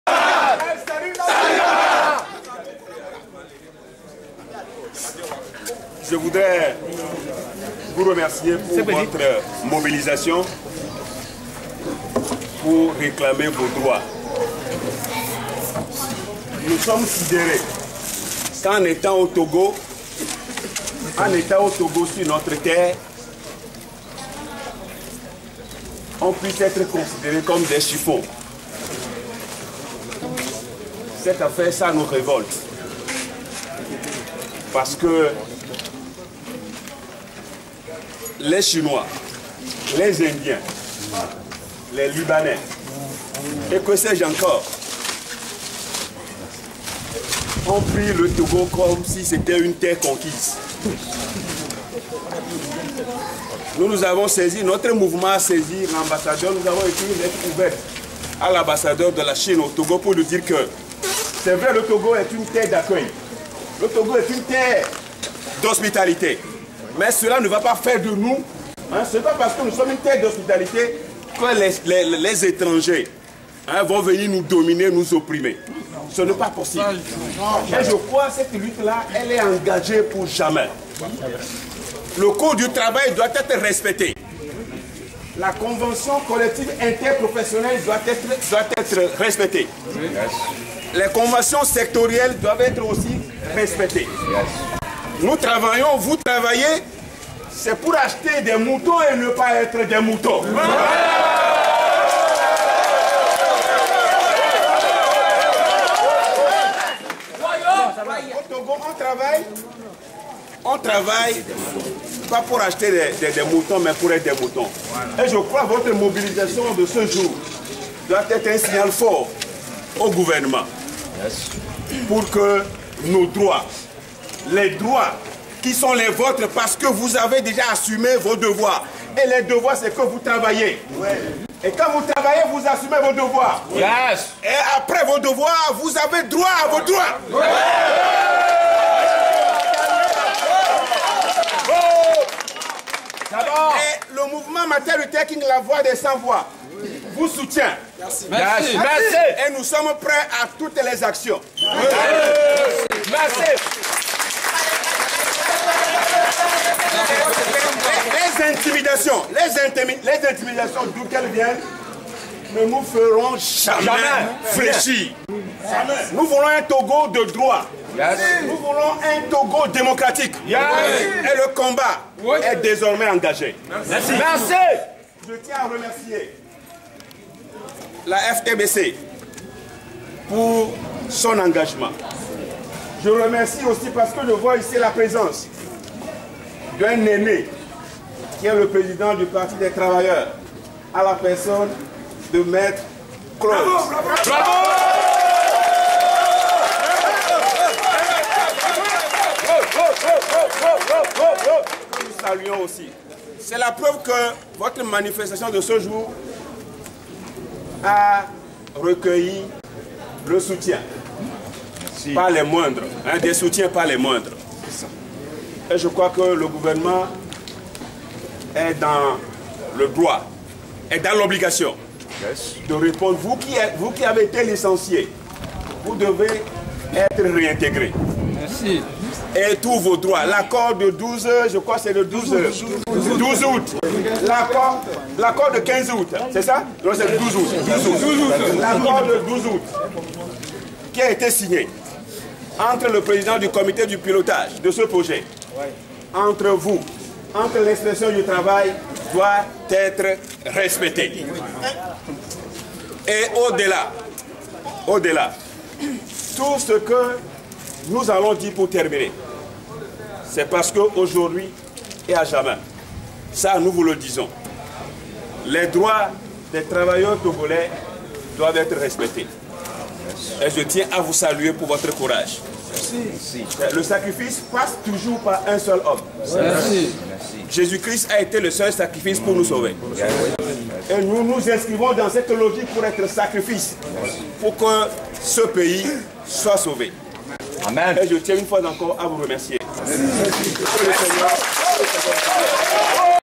Pasteur Edoh Komi:"Au Togo, on ne travaille pas pour acheter des moutons mais pour être des moutons"
12 Novembre 2011. Message de soutien du Pasteur EDOH, président du Mouvement Martin Luther King (MMLK)  aux ouvriers de la nouvelle aérogare de Lomé. Ils étaient devant le Ministère du Travail pour dénoncer les traitements inhumains et exiger la réintégration d'ouvriers abusivement licenciés. Ils avaient le soutien de Fédération togolaise du bois et de la construction (FTBC), du Parti des Travailleurs de Claude AMEGANVI et du député Alphonse KPOGO